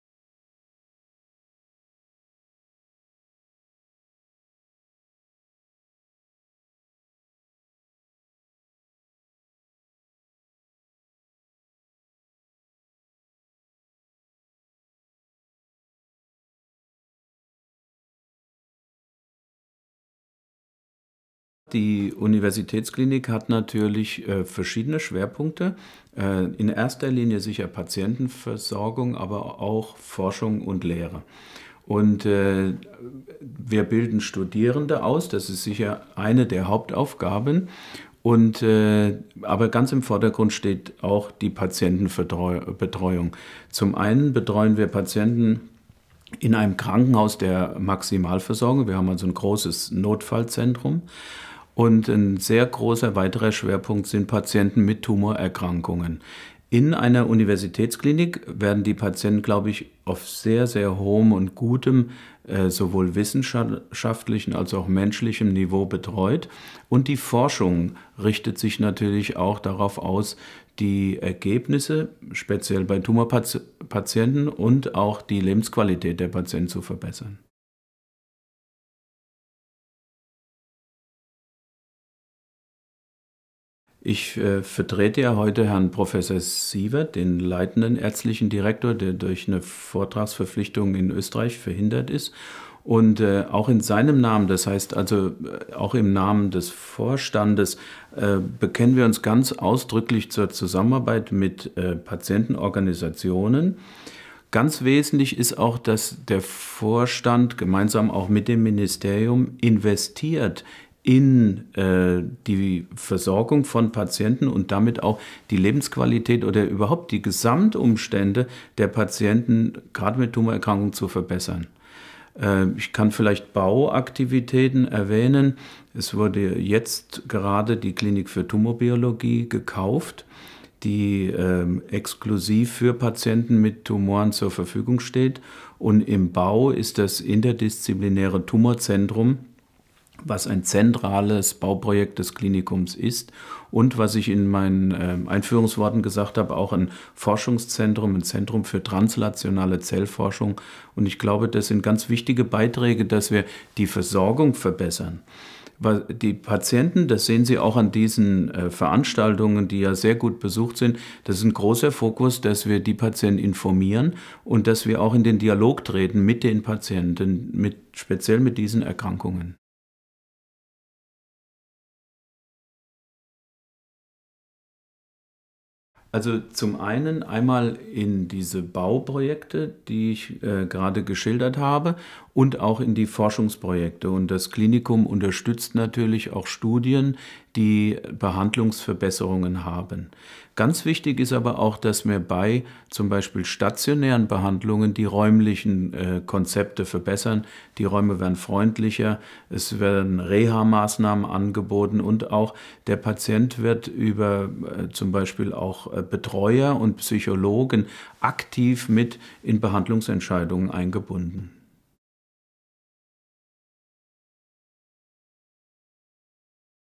Prof. Schmelzeisen: Arbeit des Uniklinikums Freiburg & Zusammenarbeit mit Patientenorganisationen
Prof. Dr. Dr. dent. Rainer Schmelzeisen, stellvertretender ärztlicher Direktor des Universitätsklinikums Freiburg, Klinik für Mund-, Kiefer- und Gesichtschirurgie, berichtet in diesem Videointerview über die Arbeit des Universitätsklinikums und die Zusammenarbeit mit Patientenorganisationen. Weiterhin legt er dar, wie das Universitätsklinikum in die Verbesserung der Behandlung von Krebspatienten investiert.